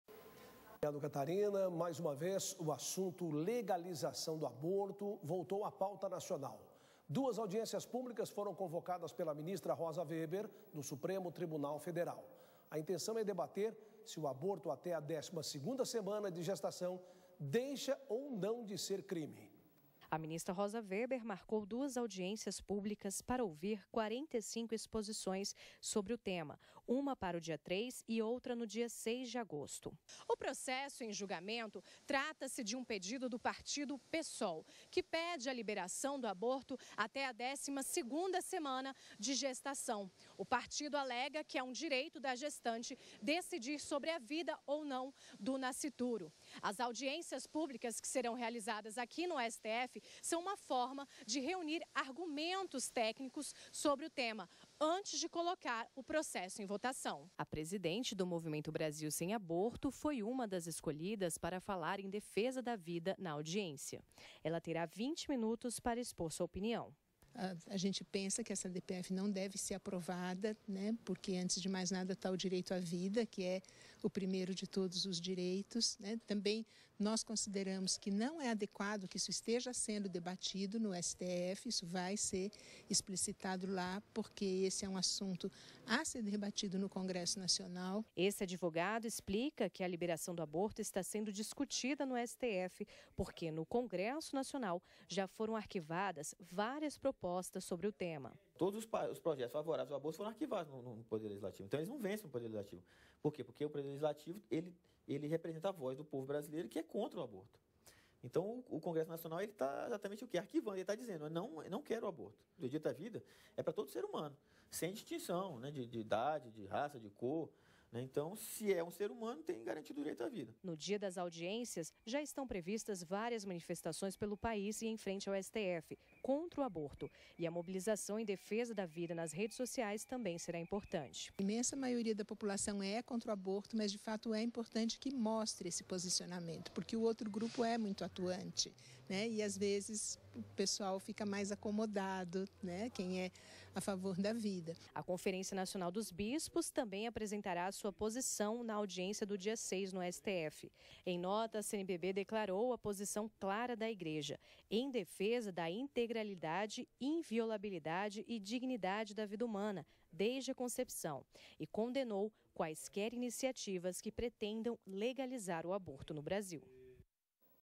Legalização do aborto volta ser pauta de discussão no STF
Mais uma vez, a legalização do aborto volta à pauta nacional.Duas audiências públicas foram convocadas pela ministra Rosa Weber, do Supremo Tribunal Federal. A intenção é debater se o aborto até a décima segunda semana de gestação deixa ou não de ser crime.

Reportagem de Carolina Curvello e Emerson Câmara.